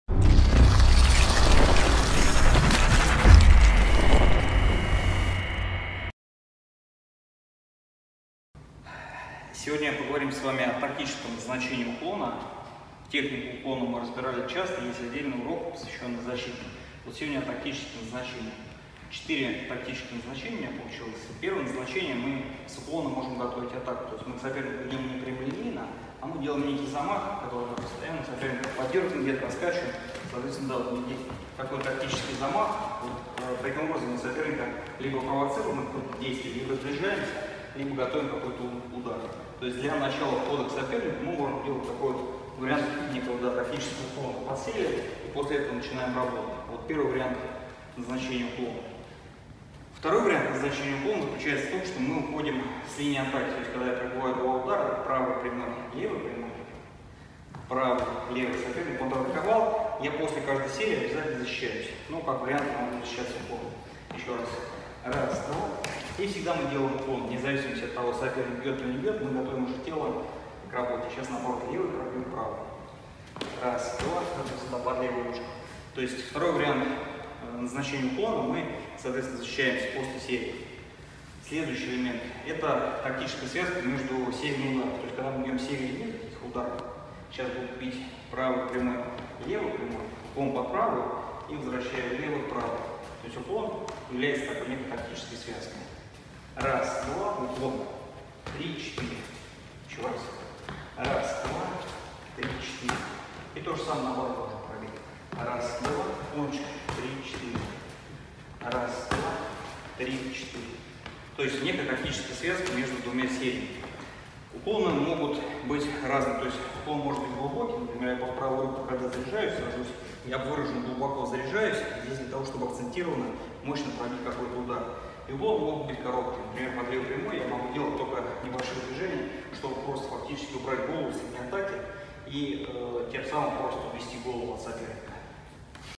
Бокс: тактическое назначение и разновидности уклона.
Анатомия бокса
Уваров Максим - тренер по боксу
+ обзоры и прогнозы прошедших и предстоящих боксерских поединков.